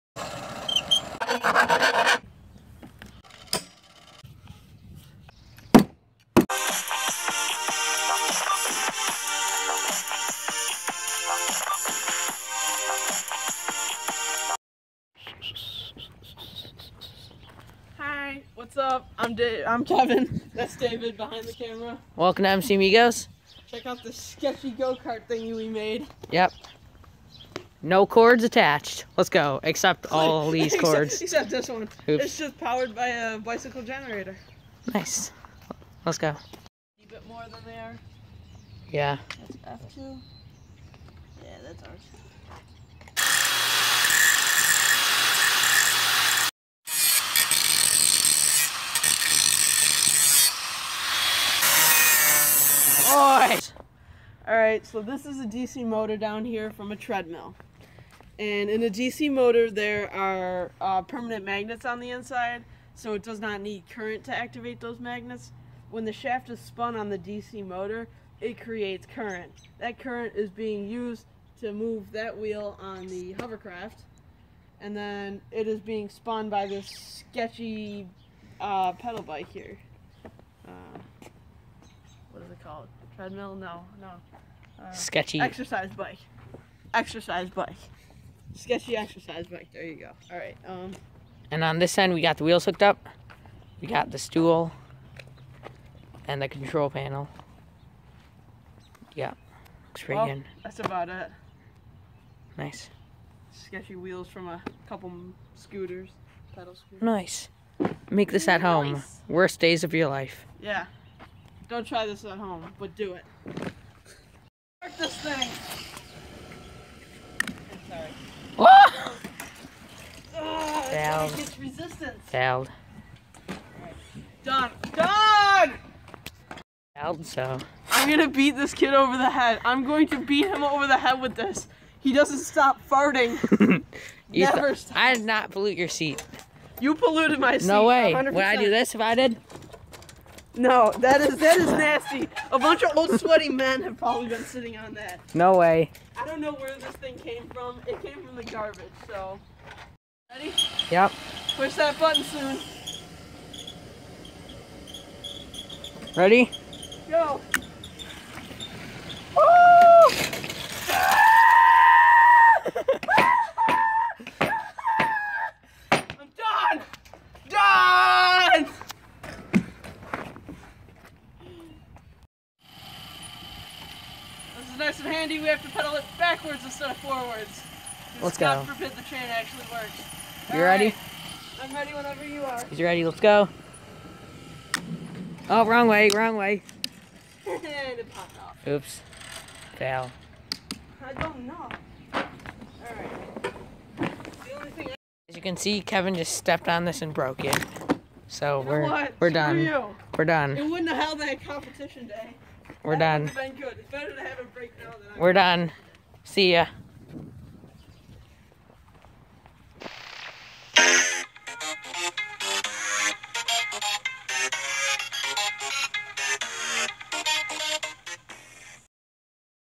Homemade generator powered Go-Kart hovercraft tutorial!!
Check out this insane project David and his buddy did... Homemade generator powered by Go-Kart hovercraft! It's pretty epic!!

We have huge plans for the future and we want you to be a part of them!

Adios Amigos
        ~The Amigo Family